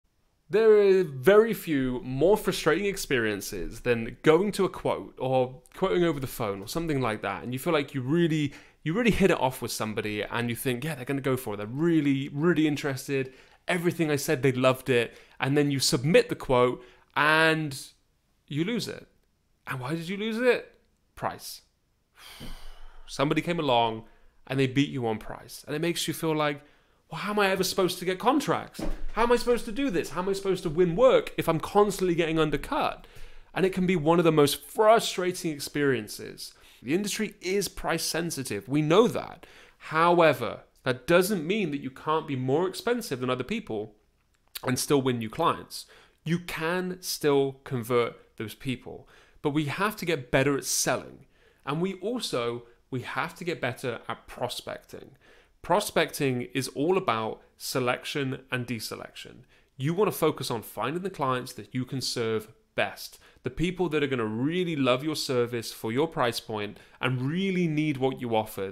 How to not be beaten on price for your cleaning quotes
Do you find you often lose a contract for your cleaning business based on the price?

There’s no worse feeling than thinking you have a great job in the bag and then after quoting them, you lose it based on the price.

Now, as annoying as this can be, there are ways to avoid this altogether without lowering your price. You need to focus on HOW you are selling to these people.

If you don't sell to them correctly, if you don't focus on understanding their pain points, and you’re too busy just telling them, “This is what we do” and you don't ask them questions about their current experience or questions about the current company and about what they're dealing with, then they're probably not going to go with you.

You need to build rapport. You need to ask great questions. You need to lead that sales meeting. You need to show the features of your service, but most importantly, you need to link that to the benefits.

So if you're in that position, if you know that you want to improve those things, drop me a message. At The Clean Up Academy, this is what we teach you, how to be a great salesperson who can take control of your business and your income.